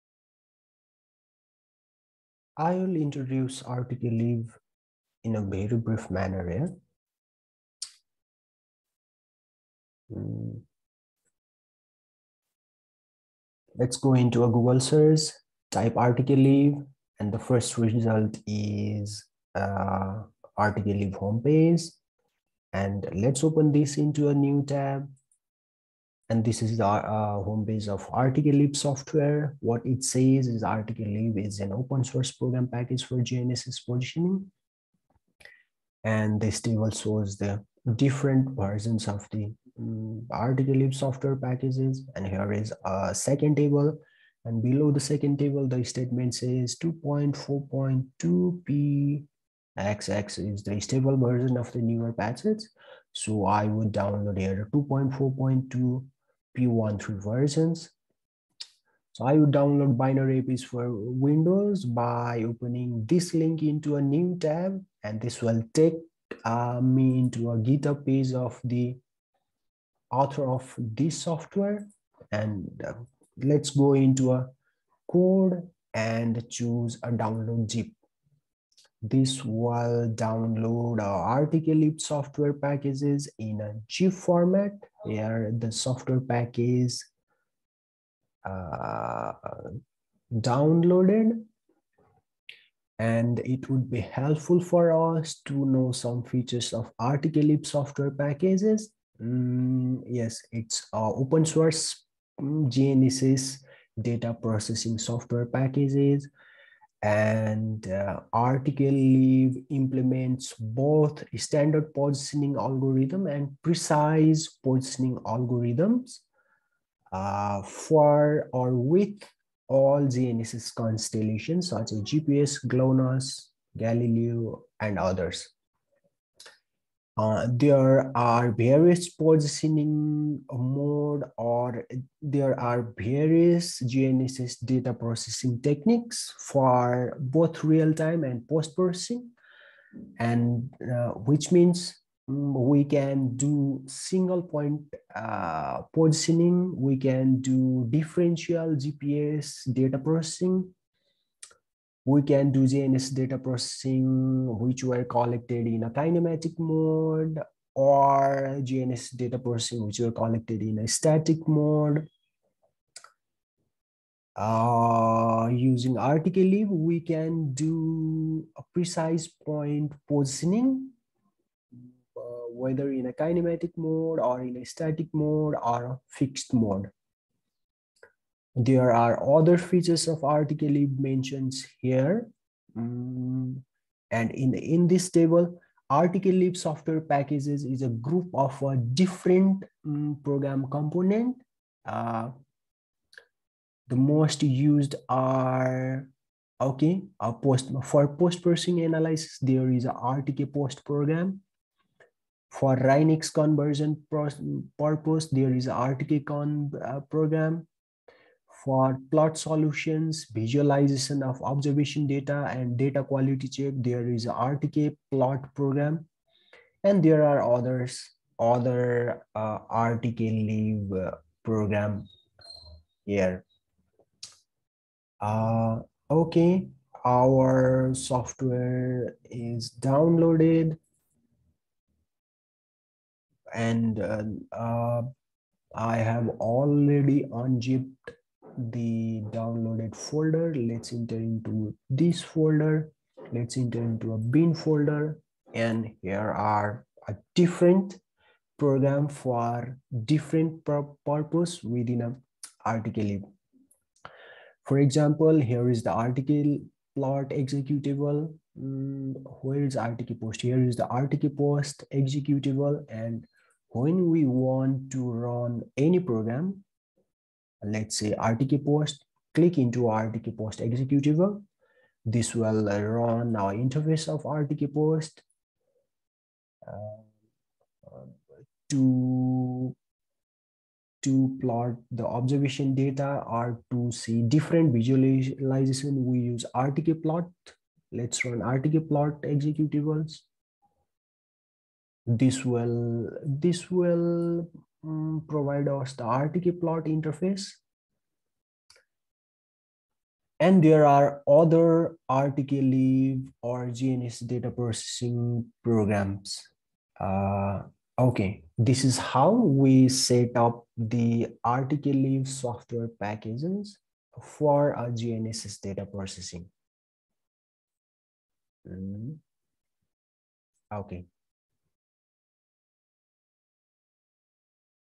A Brief Introduction of RTKLIB
The brief introduction of RTKLIB. RTKLIB is an open source program package for GNSS positioning.